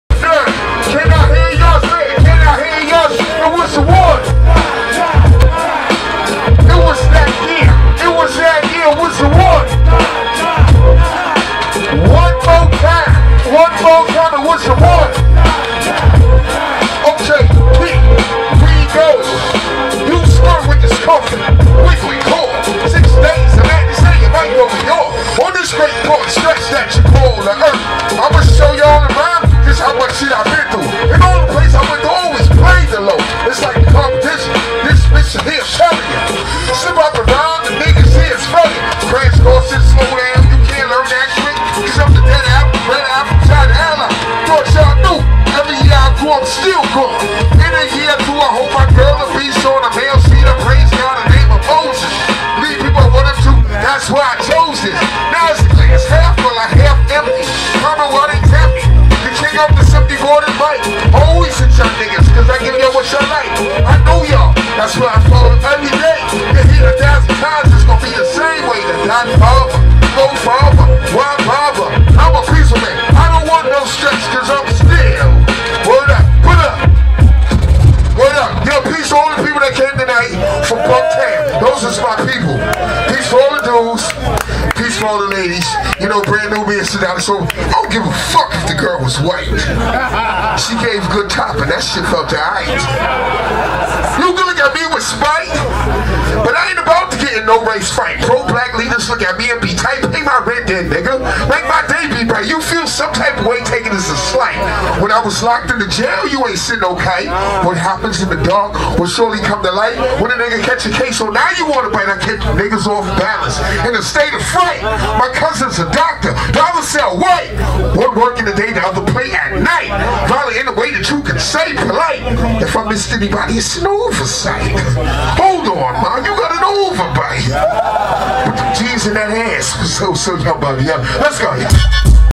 Sadat X - Bucktown USA 10yr Anniversary
Sadat X hits the stage and puts on a dope performance
Bucktown USA 10yr Anniversary

Pyramid Lounge NYC